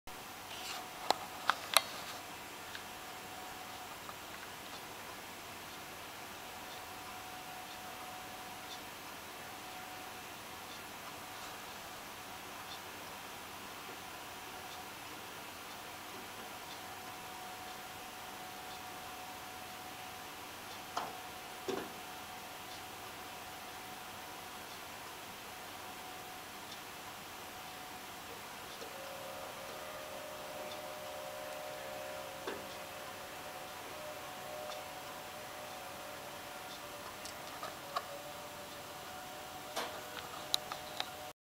Squirrel in a box | กระรอก | 松鼠 | リス
Cute red squirrel getting out of a feeder-box and asking for more walnuts.